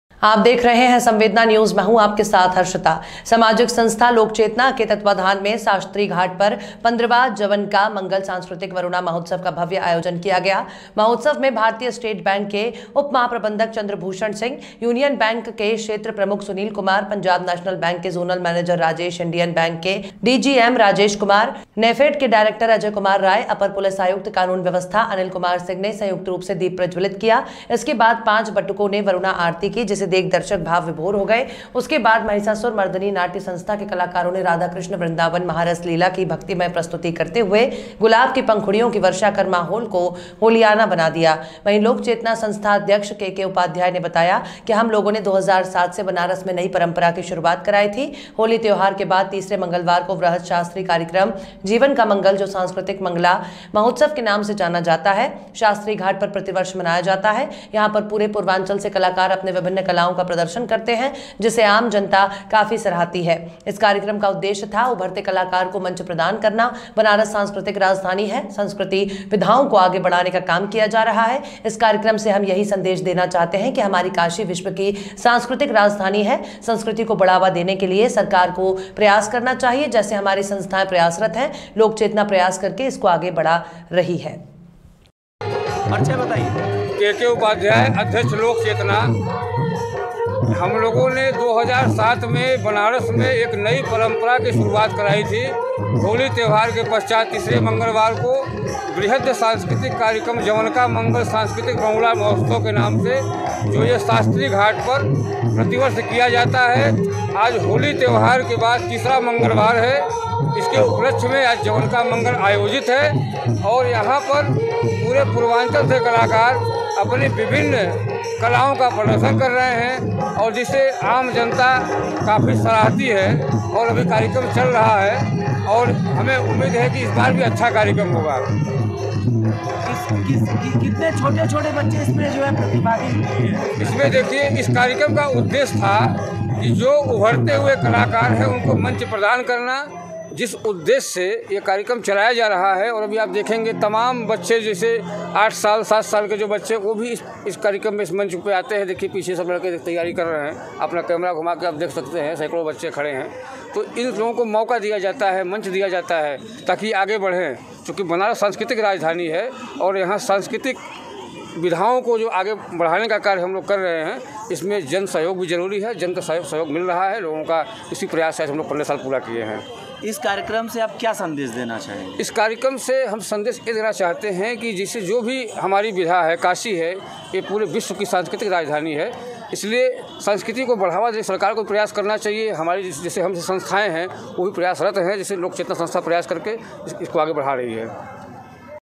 हास्य कवियों ने छोड़े हास्य-व्यंग,तीर वरुणा आरती को देख दर्शक हुए निहाल
SANVEDANA NEWS